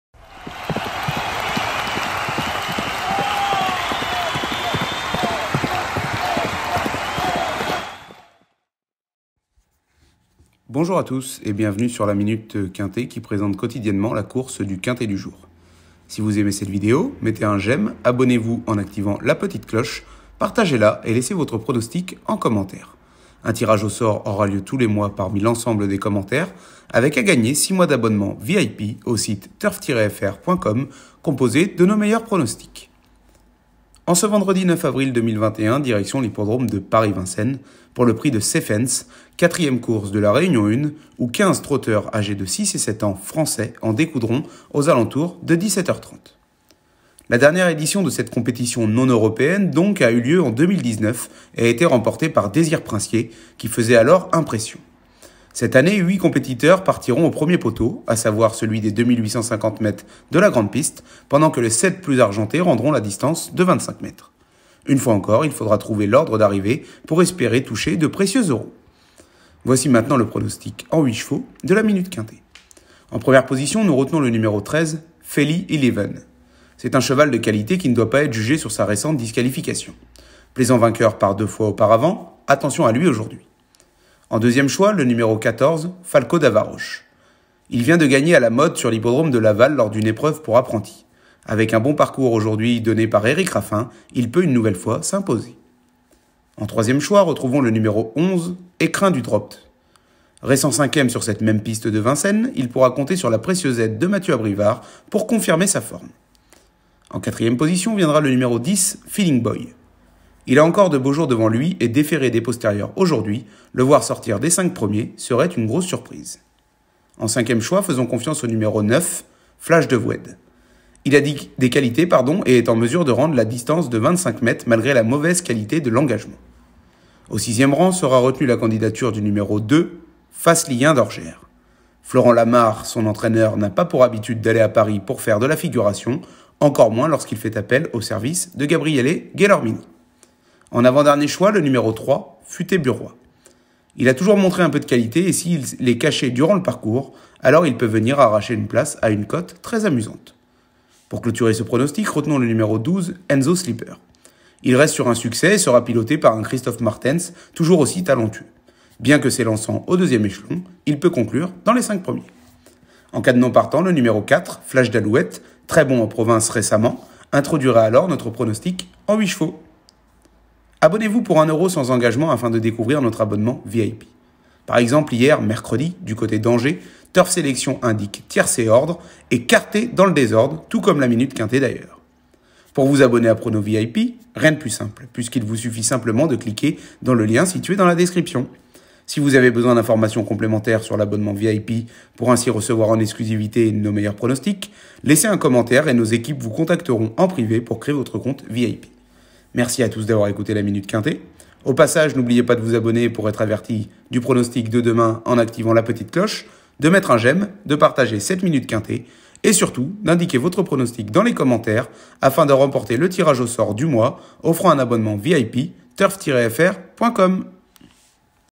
Minute Quinté TURF FR : PRIX CEPHENS - Vendredi 9 Avril 2021 - Paris Vincennes 🏇 PMU #235541
🥇 Retrouvez le pronostic exclusif de notre expert Turf Lucius Simenon pour le PRIX CEPHENS le Vendredi 9 Avril 2021 à l'hippodrome de Vincennes à Paris en France.

✅ NOS LOGICIELS TURF :
✔️ Logiciel TURF INFO Professionnel Edition: